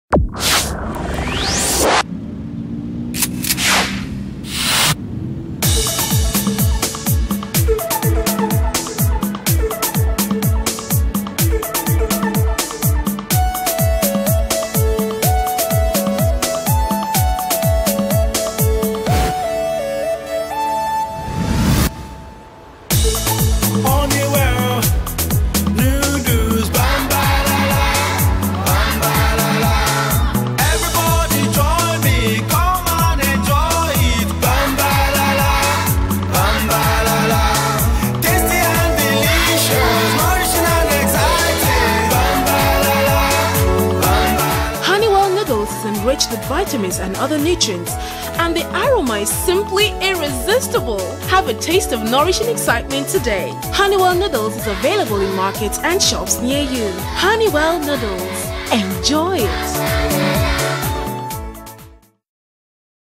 Honeywell Noodles... Bam bam la la
Honeywell Noodles TVC. Tasty and delicious, nourishing and exciting.